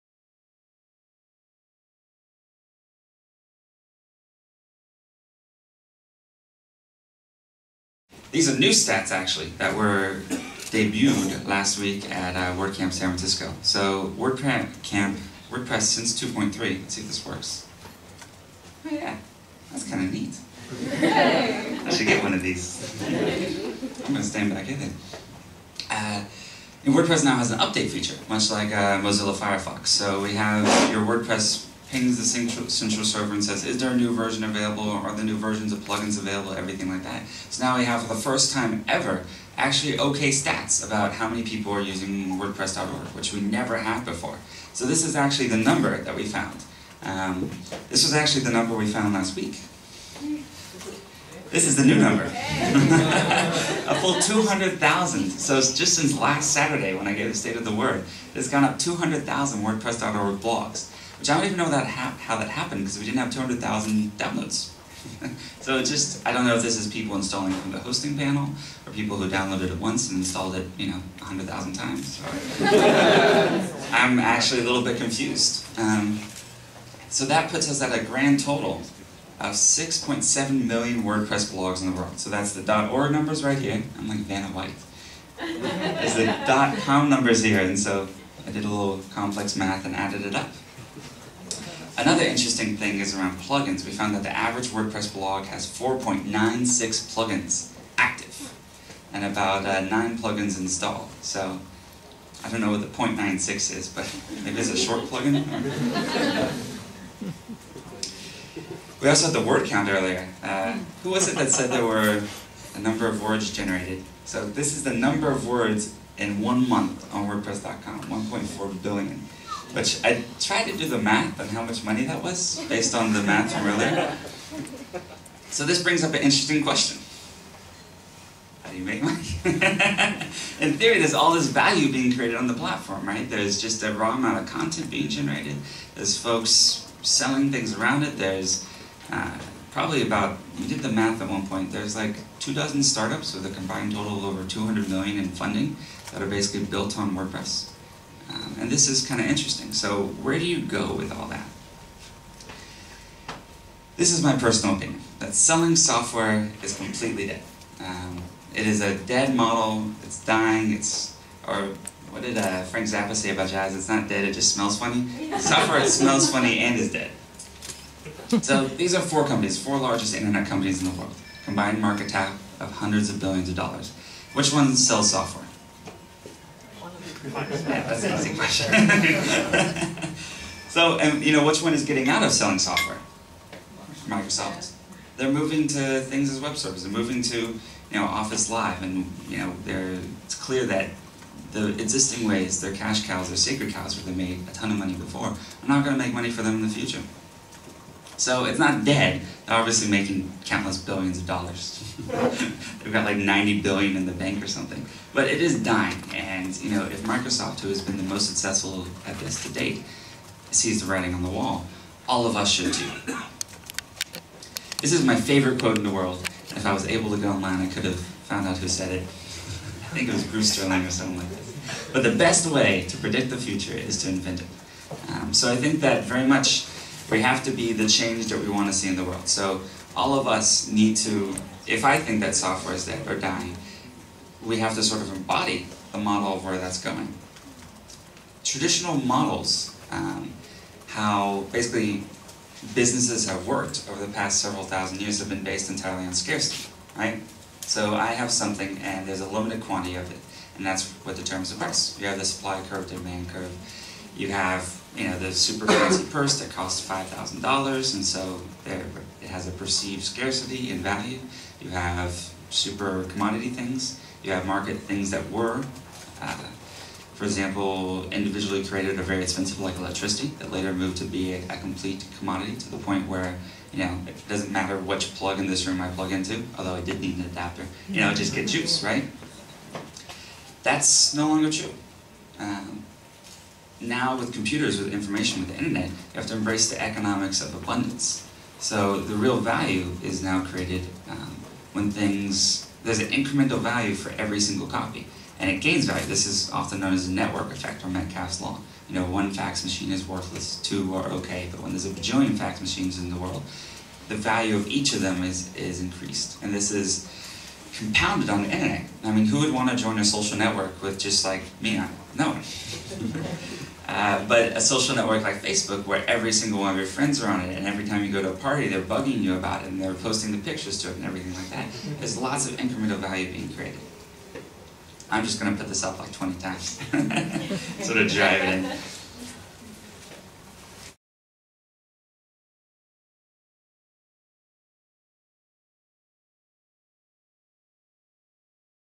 WordCamp South Africa 2008 - Matt Mullenweg
Matt Mullenweg, the founding developer of WordPress gives an amazing talk at WordCamp South Africa 2008